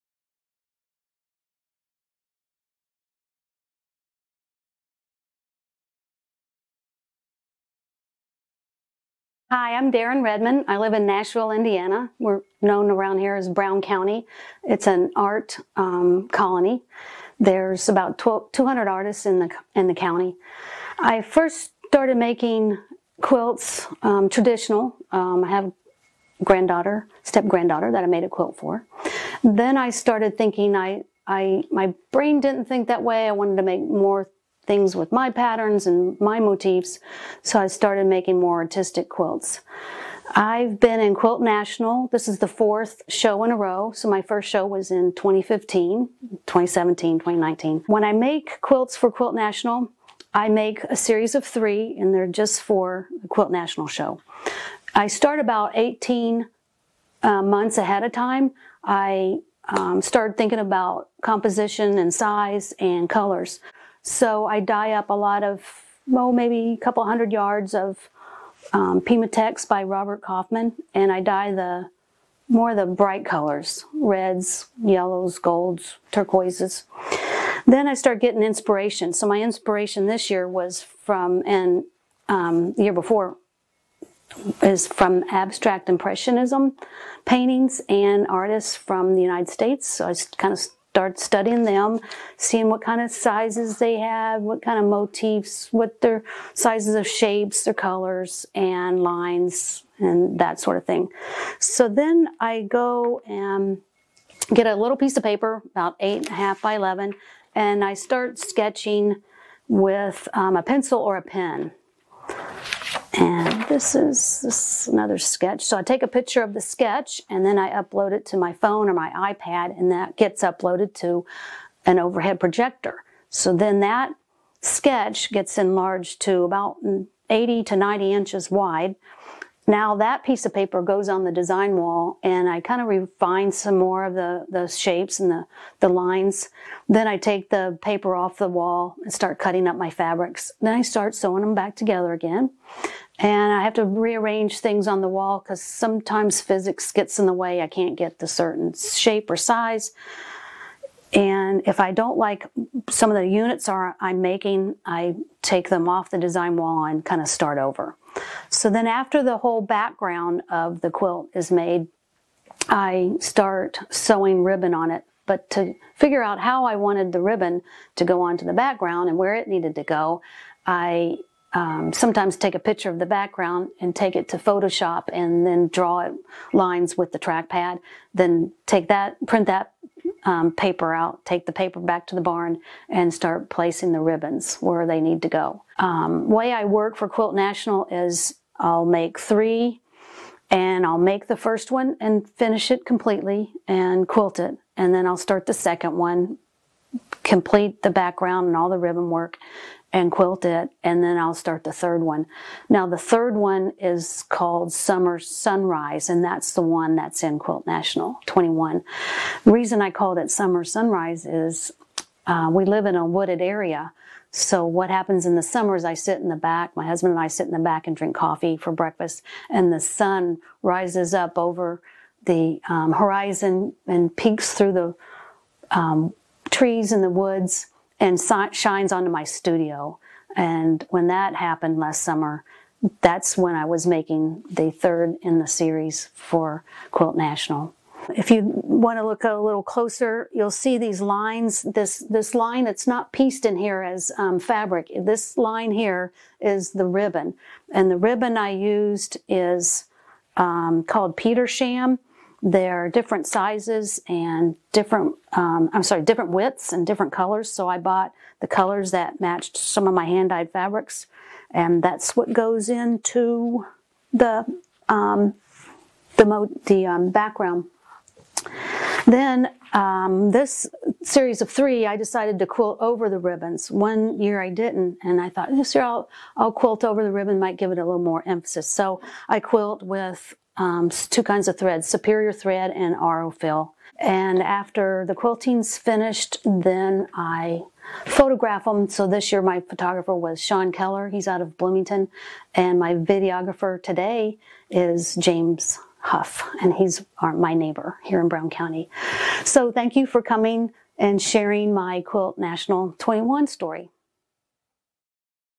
Quilt National '21 Artist Talk: Daren Redman
Quilt National ‘21 artist, Daren Redman talks about her artwork Summer Sunrise.

Quilt National is produced by The Dairy Barn Arts Center in Athens, Ohio. More information about Quilt National at dairybarn.org.